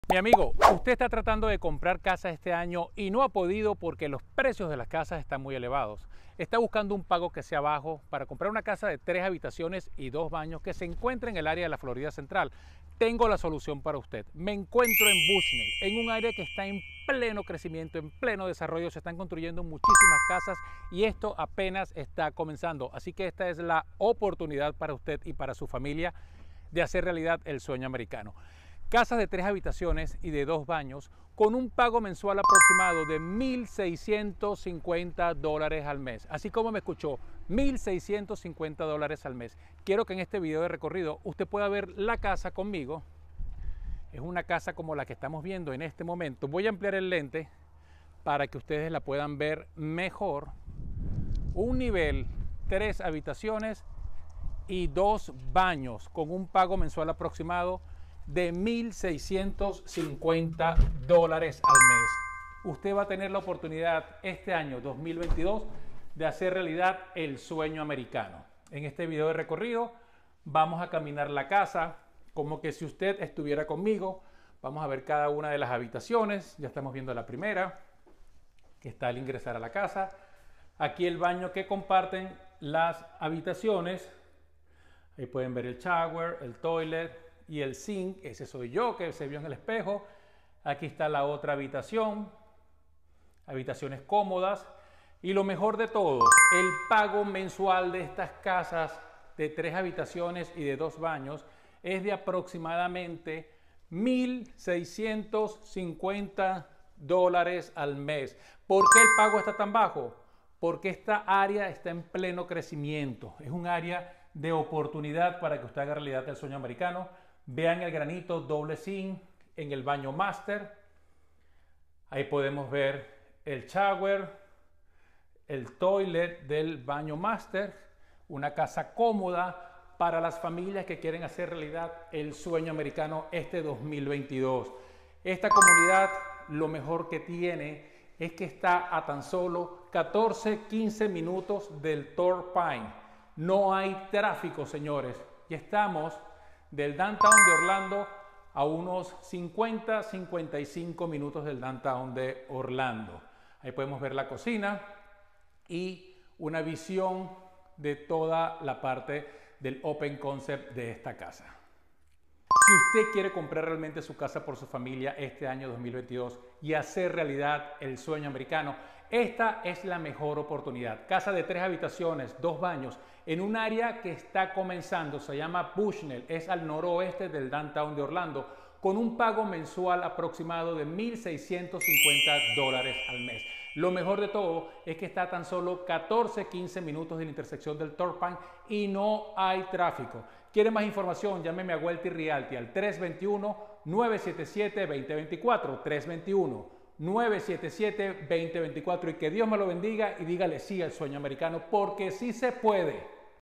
Casas NUEVAS Gran Orlando pago mensual $1,650 y ayuda gastos $6,000. HAZ REALIDAD EL SUEÑO AMERICANO
Casas NUEVAS en Gran Orlando pago mensual $1,650 y ayuda gastos $6,000. HAZ REALIDAD EL SUEÑO AMERICANO!!!

QUIERES HACER REALIDAD EL SUEÑO AMERICANO???
Te ofrecemos casas de 3 habitaciones y 2 baños con un pago mensual aprox. $1,650 y $6,000 ayuda para gastos de cierre.

La comunidad está en Bushnell, Florida al noroeste del Downtown de Orlando.

Una de las ventajas que ofrece esta área en pleno desarrollo es que está a tan solo a 14-15 minutos del Turnpike, la vía principal que atraviesa la Florida y que adicionalmente en esta área NO HAY TRÁFICO!!!